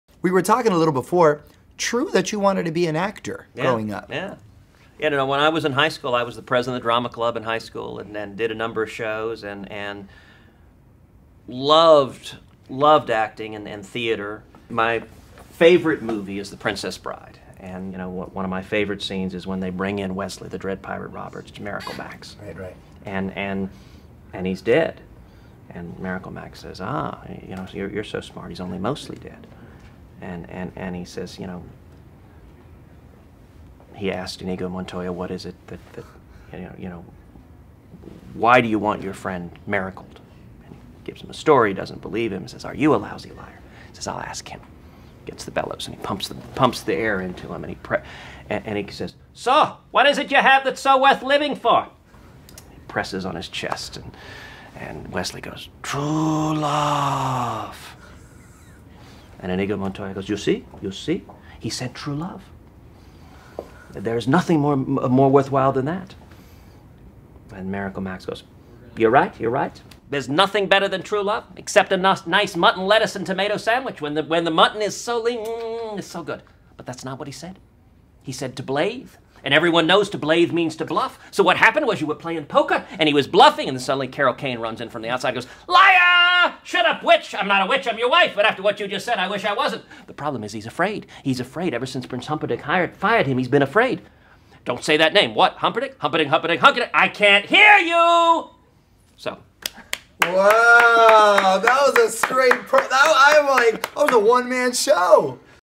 Watch Ted Cruz Re-enact a Scene from 'The Princess Bride'
"Extra's" Mario Lopez recently sat down with GOP presidential candidate Senator Ted Cruz, and it turns out that the Texas senator was head of his drama club in high school and is a huge fan of "The Princess Bride."

Naturally, we had to ask the presidential hopeful if he could re-enact a scene from the movie. Take a look!